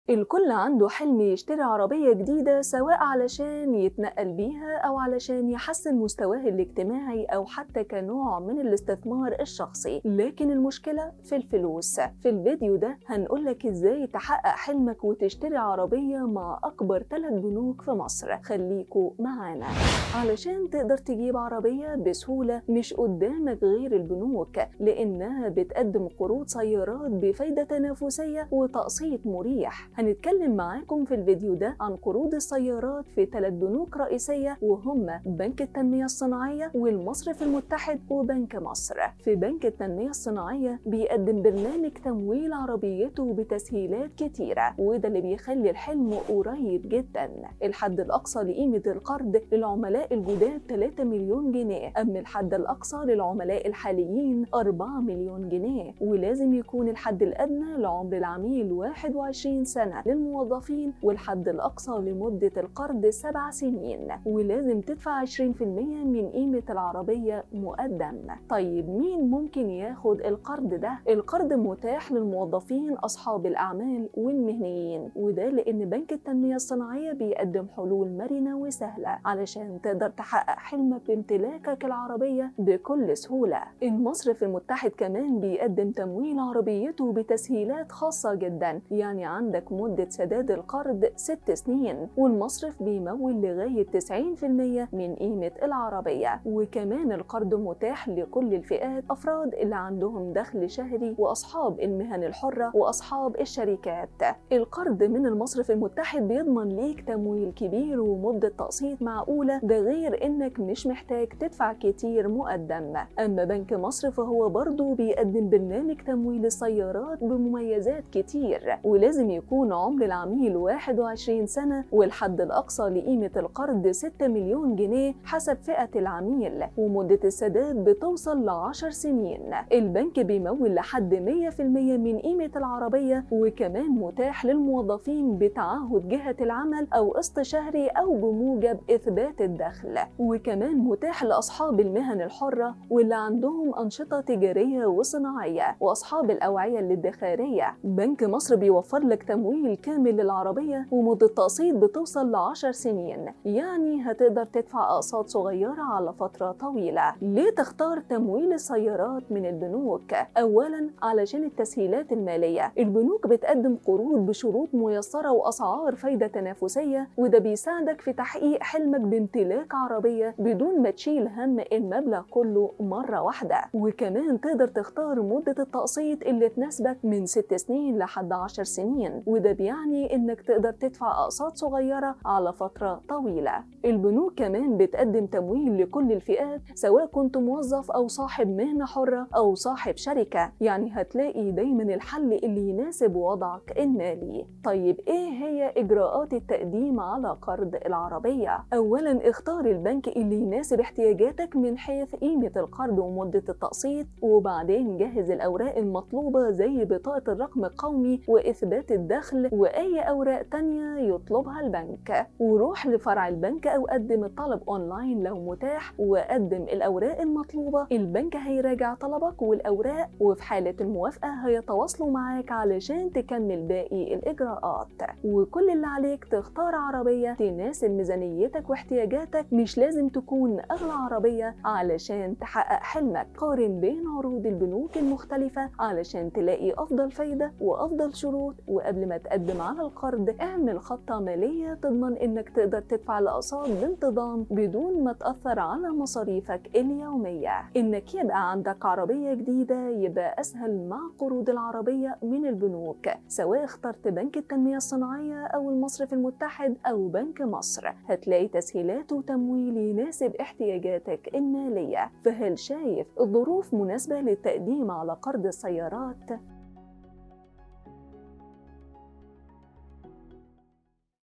أولهم بنك مصر.. 3 بنوك تقدم قرض السيارة لمدة سداد طويلة تصل إلى 10 سنين.. وحد أقصى 6 مليون جنيه:
الكل عنده حلم يشتري عربية جديدة، سواء عشان يتنقل بيها أو علشان يحسن مستواه الاجتماعي أو حتى كنوع من الاستثمار الشخصي… لكن المشكلة في الفلوس